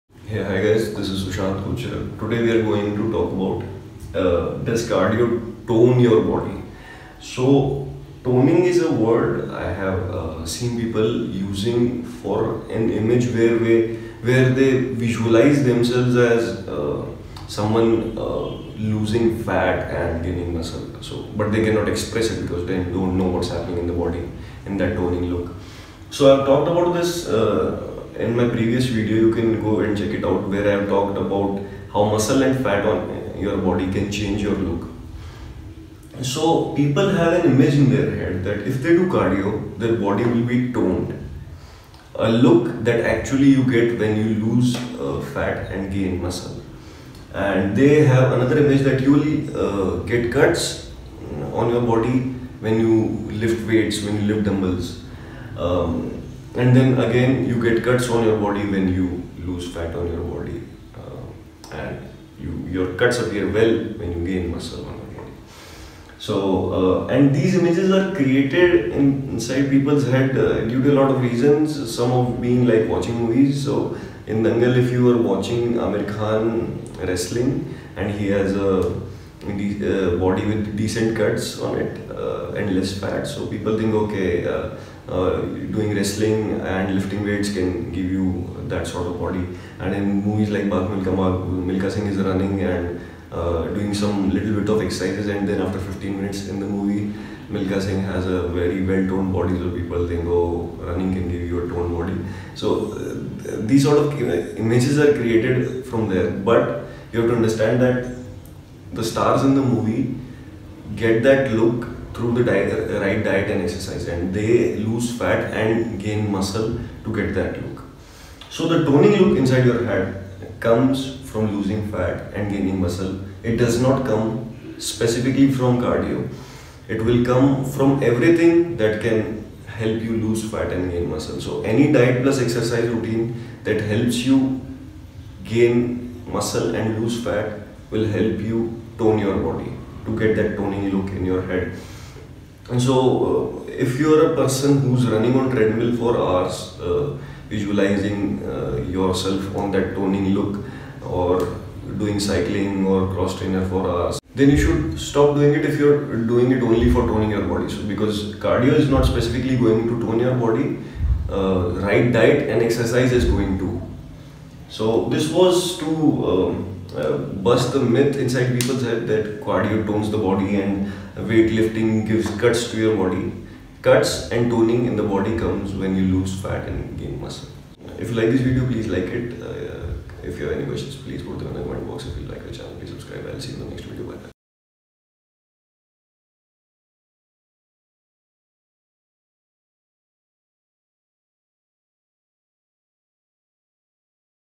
Does cardio 'tone' your body ? (English)
Video talks about if cardio tones your body. If cardio adds that toned look to your body. Answer is no - cardio won't do the toning. Gaining muscle & losing fat will. It is English version of the video.